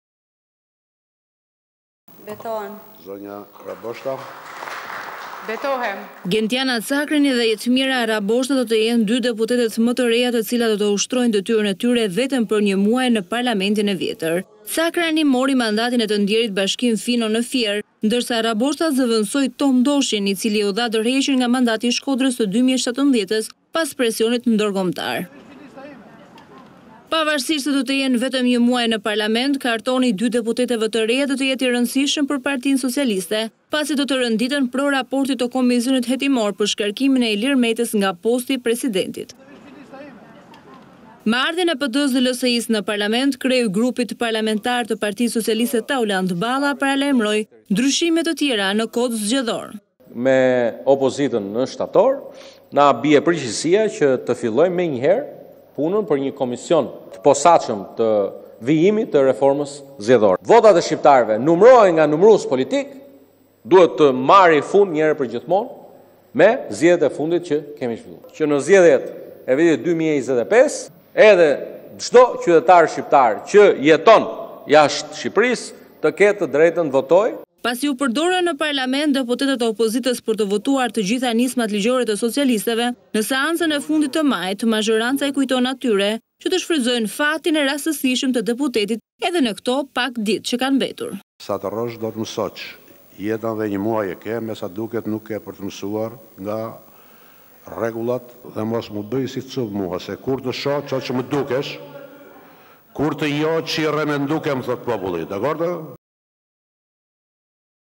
PS paralajmëron reformë të re zgjedhore në shtator
Gentiana Cakrani dhe Jetmira Rraboshta do të jenë dy deputetët më të reja të cilat do të ushtrojnë detyrën e tyre vetëm për një muaj në Parlamentin e vjetër.

Cakrani mori mandatin e të ndjerit Bashkim Fino në Fier, ndërsa Rraboshta zëvendësoi Tom Doshin i cili u dha dorëheqjen nga mandatin e Shkodrës të 2017 pas presionit ndërkombëtar.

Pavarësisht se do të jenë vetëm 1 muaj në Parlament, kartoni i dy deputetëve të reja do të jetë i rëndësishëm për PS, pasi do të renditen pro raportit të Komisioni hetimor për shkarkimin e Ilir Metës nga posti i Presidentit.

Me ardhjen e PD dhe LSI në parlament, kreu i grupit parlamentar të PS Taulant Balla paralajmëron ndryshime të tjera në Kod Zgjedhor.

"Me opozitën në shtator në bie përgjegjësia që të fillojmë menjëherë punën për një komision të posaçëm të vijimit të reformës zgjedhore.  Votat e shqiptarëve që numërohen nga numërues politikë, duhet të marrin fund një herë e përgjithmonë me zgjedhjet e fundit që kemi zhvilliar, që në zgjedhjet e vitit 2025 edhe çdo qytetar shqiptar që jeton jashtë Shqipërisë të ketë të drejtën të votojë," tha Balla.

Pasi u përdoren në Parlament deputetët e opozitës për të votuar të gjitha nismat ligjore të socialistëve, në seancën e fundit të majit, mazhoranca i kujton atyre që të shfrytëzojnë fatin e rastësishëm të deputetit në këto pak ditë.

"Sa të rrosh do të mësosh, jetën dhe një muaj e ke mesa duket nuk ke për të mësuar nga rregullat dhe mos mu bëj si cub! Kur të shoh çoç më dukesh, kur të njoh çirrem e ndukem thotë populli," u shpreh Ruçi